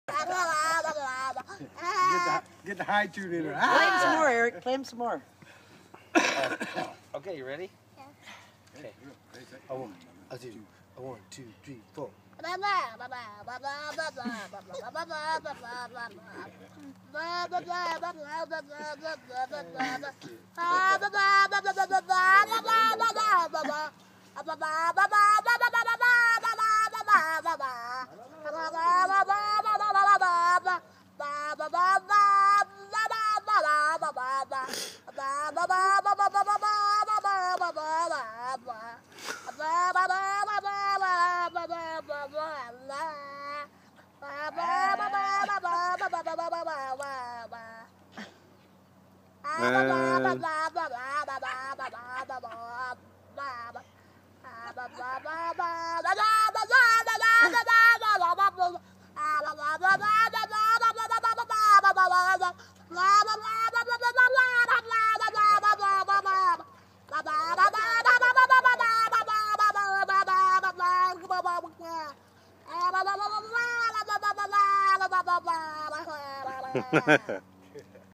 PolishEaster Music
My Brother playing some music with his musical son, Zenon